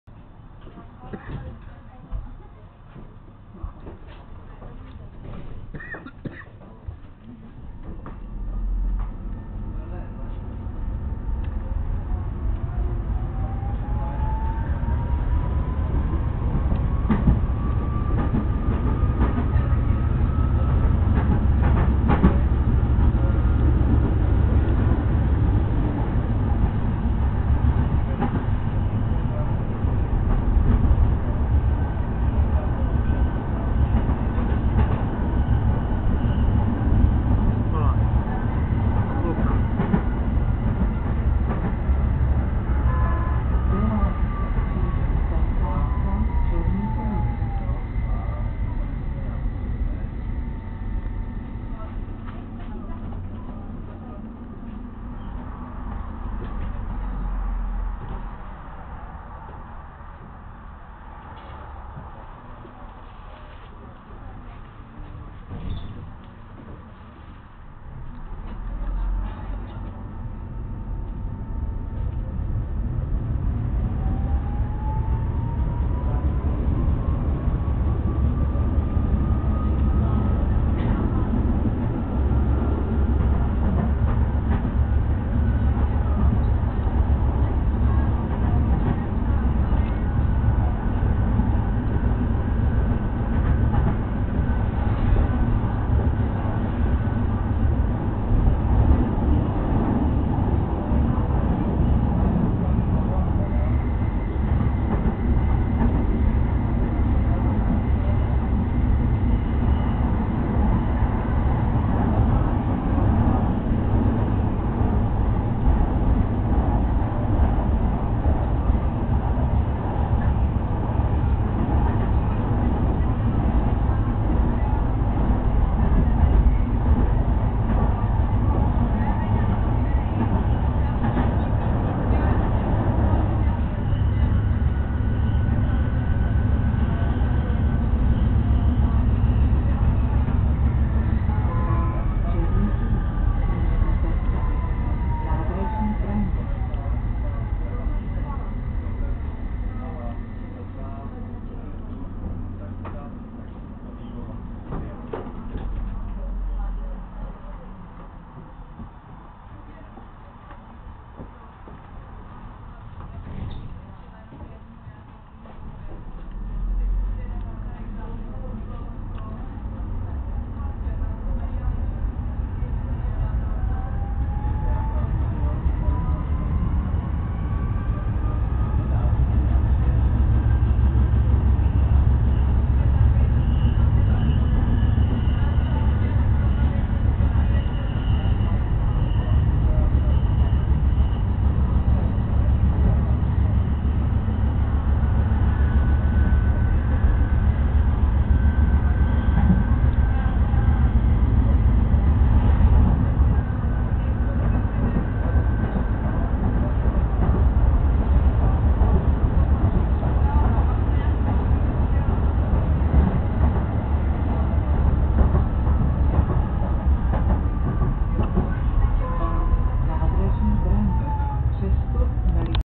tram praha V
Jízda tramvaje na nádraží Braník